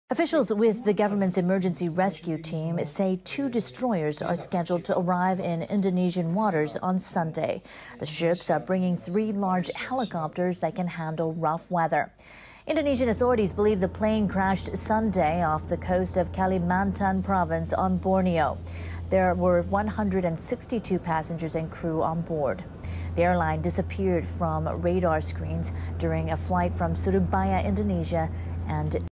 Japanese ships to join AirAsia search
Japan's Maritime Self Defense Force is sending 2 of its ships to join the search for the AirAsia jet that went missing off Indonesia.
An advance group for the Japanese government's emergency rescue team held a news conference in Jakarta on Thursday.
The group said 2 Japanese destroyers, now docking at a port in Malaysia, are scheduled to arrive in Indonesian waters as early as Sunday. They are bringing 3 helicopters.
Air traffic controllers lost contact with an AirAsia passenger jet bound for Singapore with 162 passengers and crew on board after it took off from the Indonesian city of Surabaya on Sunday.
Indonesian authorities believe the plane crashed near Kalimantan Island.
The authorities have recovered the bodies of 9 people. But search crews are struggling with bad weather. It hampers helicopters carrying divers from taking off from ships.
The officials want to continue their search with the help of the 3 Japanese helicopters. These are able to take off more easily because the Japanese vessels are larger than Indonesian ones and more stable in rough seas.